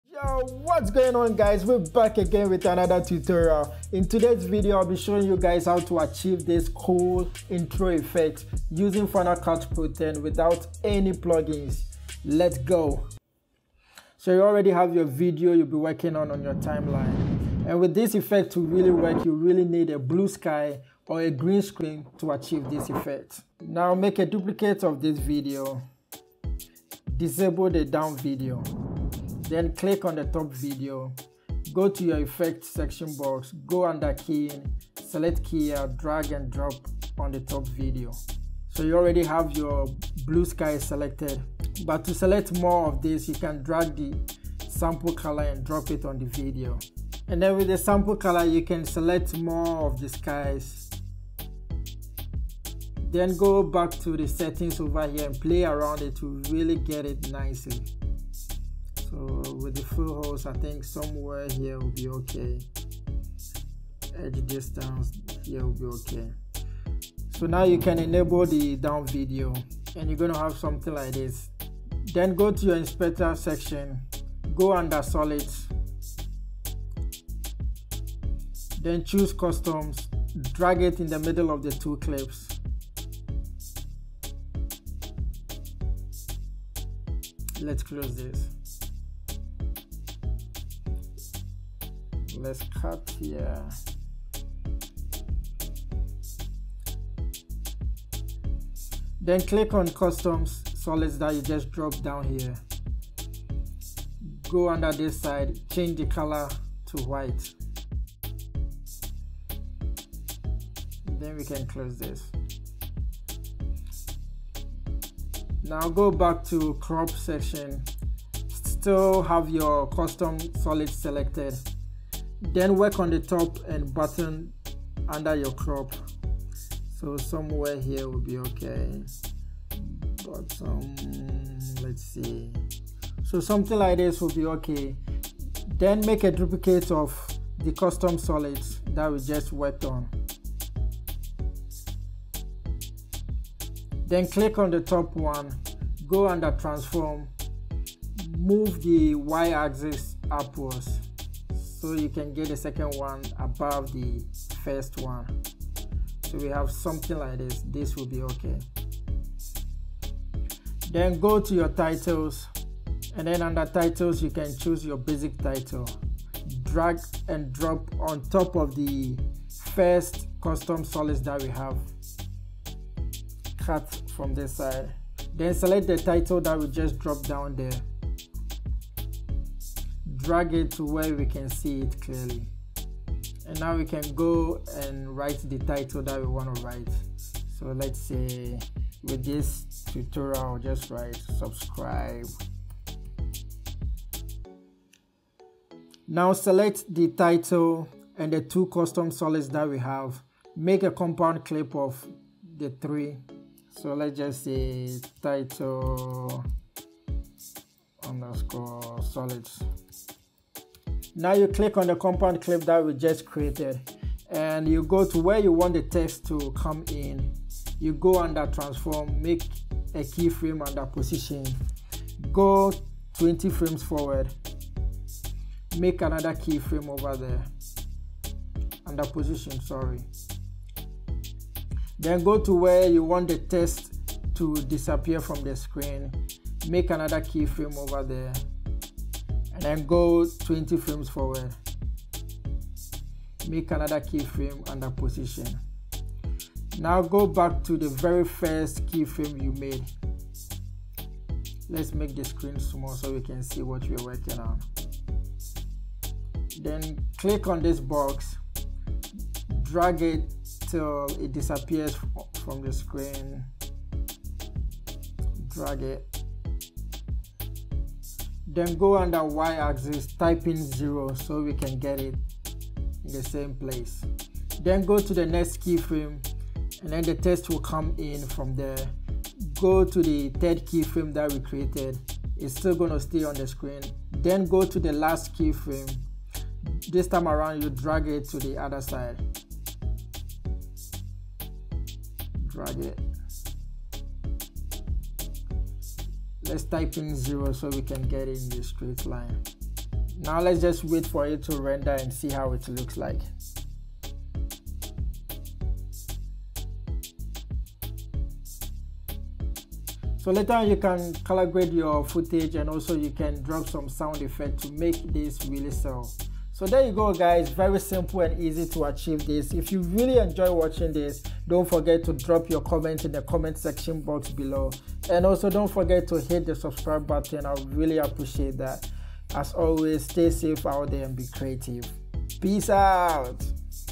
Text Title Intro Effect in FINAL CUT PRO - No Plugins
In today's video, I will be showing you guys how to achieve this cool text intro effect for your videos using final cut pro X (FCPX).

00:00​​ - Intro

00:13 - How to achieve text intro effect for your videos using final cut pro X

5:09 - Outro

Want to see more tutorials